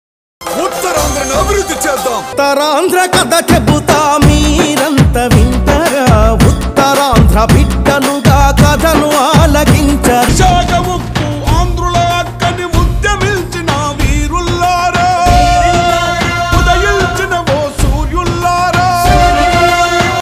గద్దర్ – గీతం కాదు, ఉద్యమం!
ప్రతి పాట వెనుక ఒక న్యాయపోరాటం, ప్రతి పదం వెనుక ప్రజల కోసం దహించే ఆవేదన. ఆయన గాత్రం ప్రజల గుండెల్లో చైతన్యాన్ని నింపి, ఉద్యమాలకు ప్రేరణగా నిలిచింది.

Share with a friend 🤝 who is a movie lover❤️‍🔥
Follow us @ukku_satyagraham for more updates